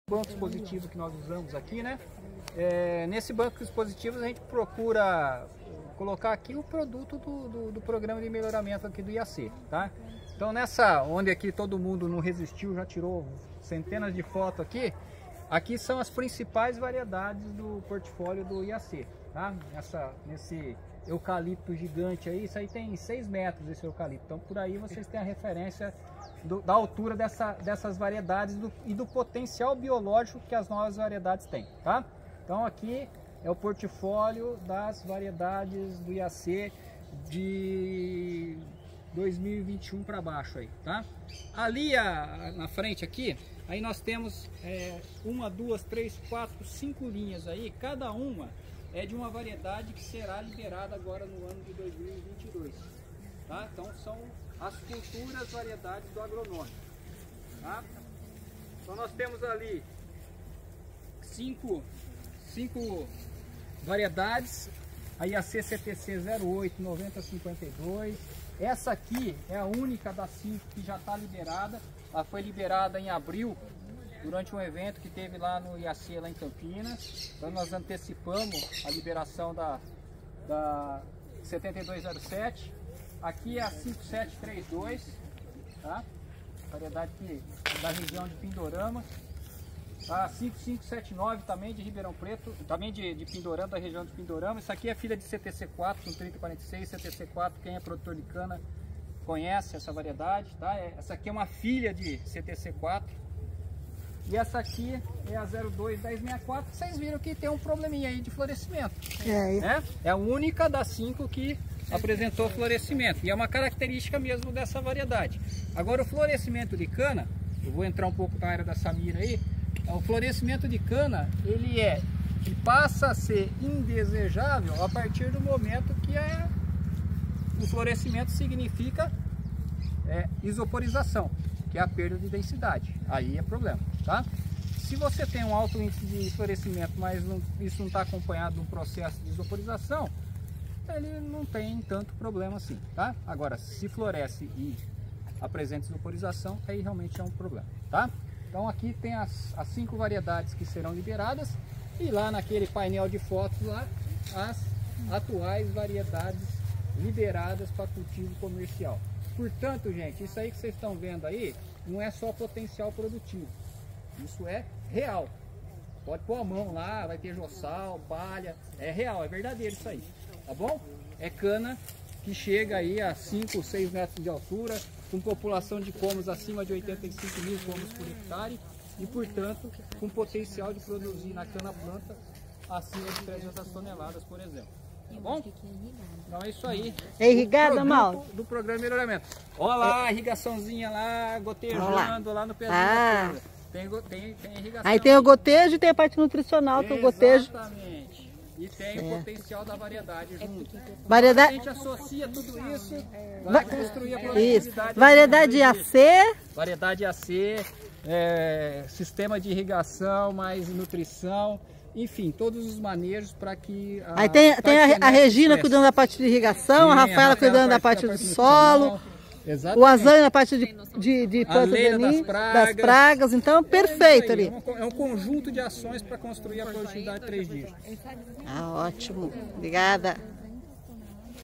Cana com 6 metros de altura, com 85 mil colmos por hectare e cana-planta acima de 300 toneladas/ha
Integrantes da visita técnica Cana Substantivo Feminino no Centro de Cana do IAC se encantam com as canas gigantes que estão no Jardim Varietal

Em visita técnica Cana Substantivo Feminino no Centro de Cana do IAC, em Ribeirão Preto, SP, realizada em 30 de junho, quando os participantes chegaram no Jardim Varietal conheceram novas variedades de cana desenvolvidas pelo Instituto Agronômico (IAC) que foram lançadas a partir de 2021 e as novas variedades que brevemente serão lançadas.

O pesquisador Mauro Alexandre Xavier, especialista em melhoramento de cana e diretor do Centro de Cana do IAC, explicou as características de cada variedade e seu potencial produtivo e a importância do manejo correto, começando pelas variedades de cana, passando pela nutrição e até a irrigação. O que resultando em canas com cinco a seis metros de altura, com população de 85 mil colmos por hectare (ha) e com potencial de produção em cana-planta acima de 300 toneladas/ha.

Confira o vídeo: